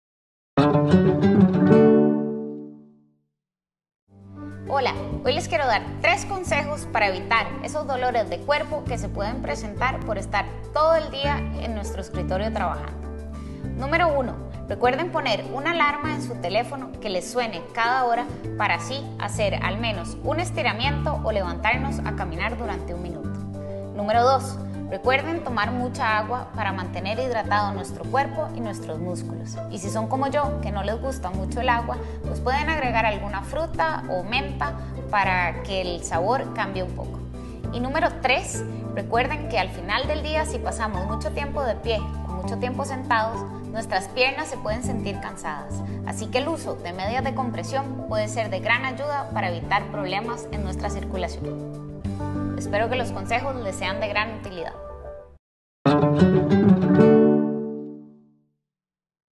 ¡3 Consejos para mejorar tu home office que necesitas conocer!
¿Quién dijo que el home office no es cansado? 💻🤔

Te mostramos 3 consejos para que no sufras dolores al estar tanto tiempo sentad@ frente a la computadora 💡